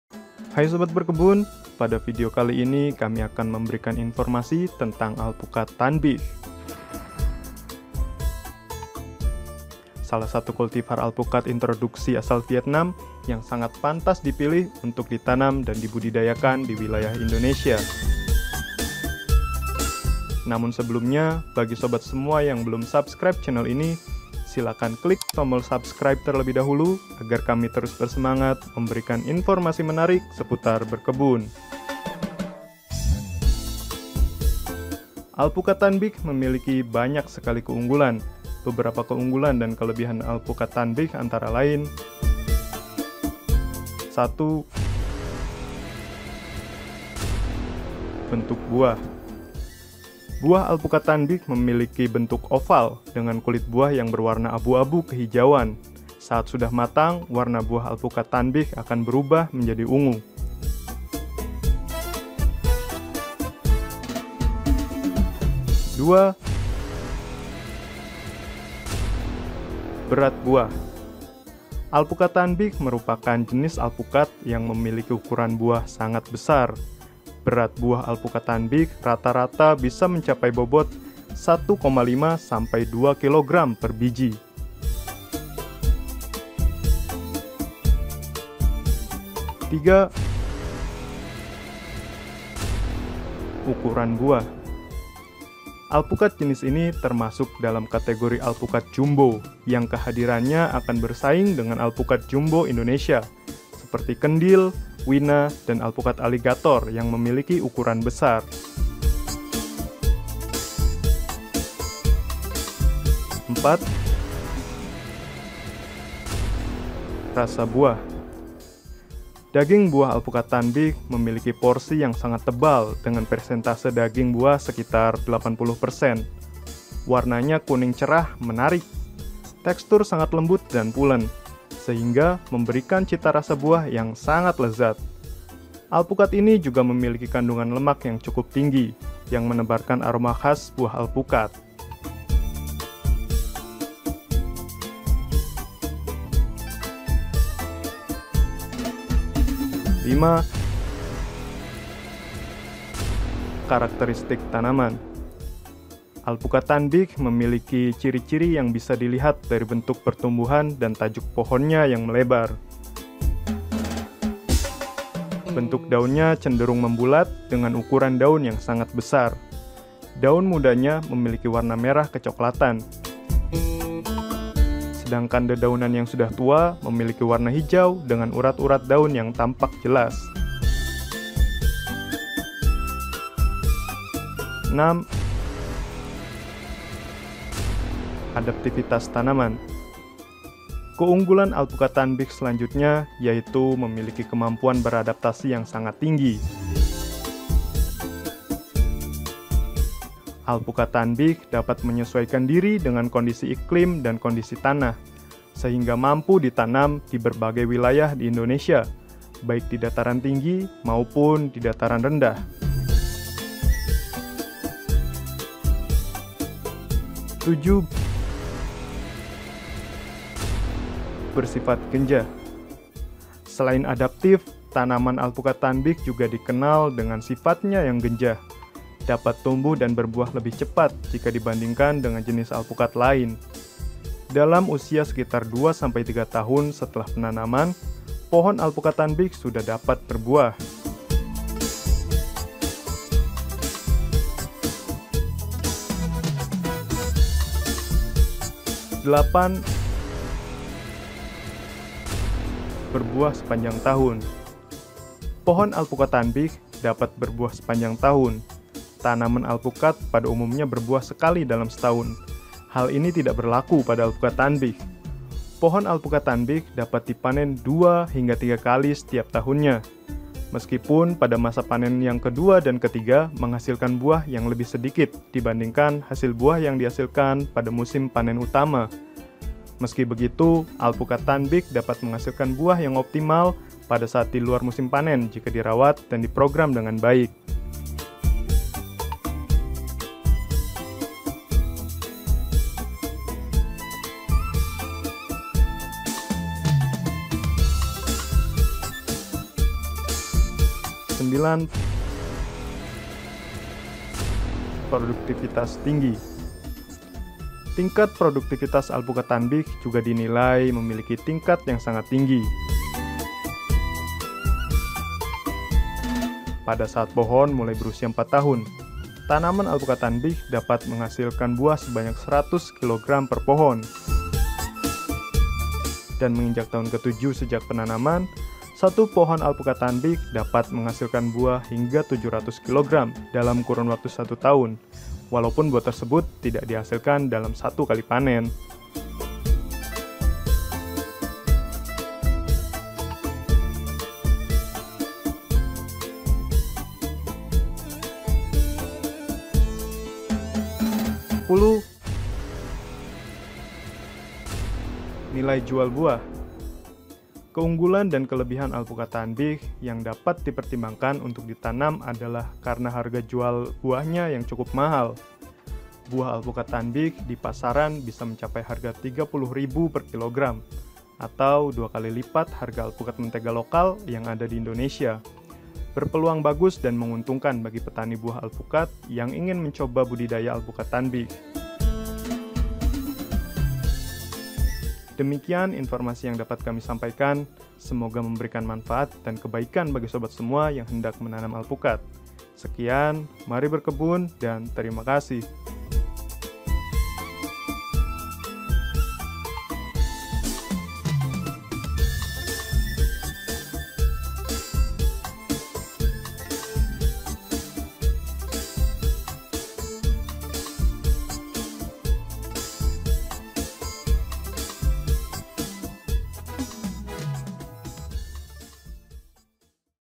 10 kelebihan dan keunggulan alp introduksi vietnam tanh bich yang perlu diketahui sebelum menanam
Kontak Bisnis kami

alpukat introduksi asal vietnam, sangat cocok untuk di budidayakan di indonesia, selain cuaca dan iklimnya yang sama, beberapa jenis alpukat asal vietnam harus menyesuaikan dengan kondisi tanah di negara kita,
salah satunya alpukat thanh bich, yang memeiliki banyak keunggulan dan kelebihan, agar alpukat than bich bisa maksimal di indonesia, kenali ciri-cirinya dan karakter fisik tanaman yang sesuai dengan elevasi untuk menanam.
alpukat vietnam berukuran jumbo, rasa enak, produktifitas tinggi, genjah, dan memiliki nilai jual yang mahal, dan prospek untuk di kebunkan.